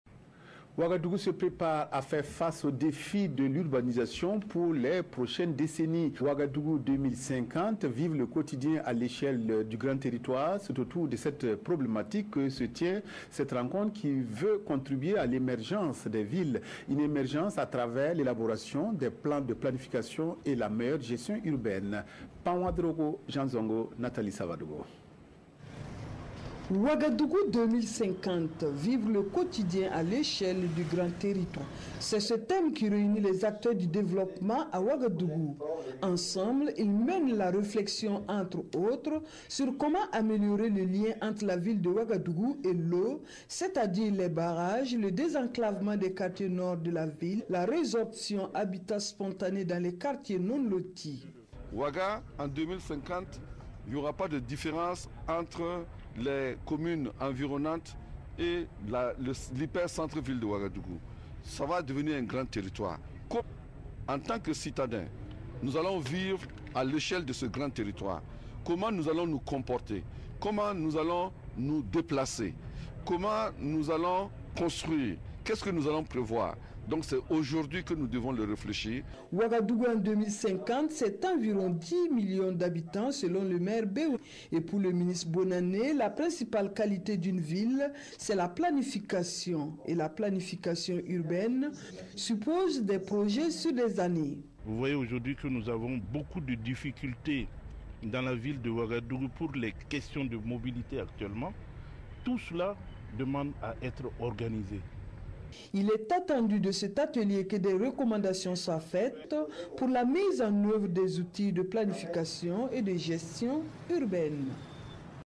RTB/Rencontre entre les acteurs du développement sur le thème Ouagadougou 2050
Rencontre entre les acteurs du développement sur le thème Ouagadougou 2050